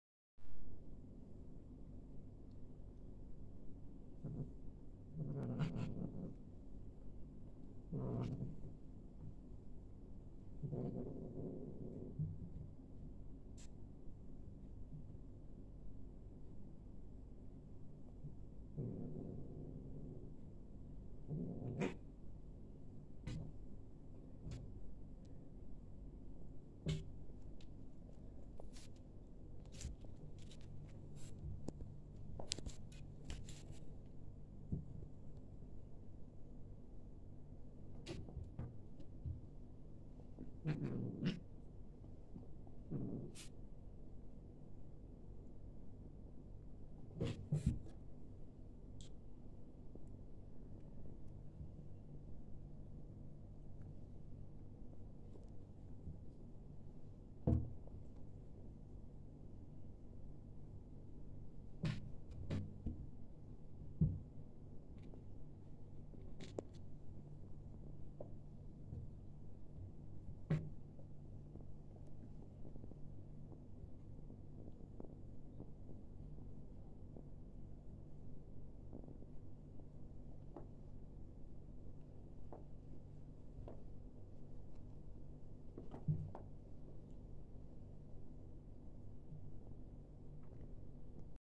Banana vs Gorilla
My cats Banana and Gorilla just playing around.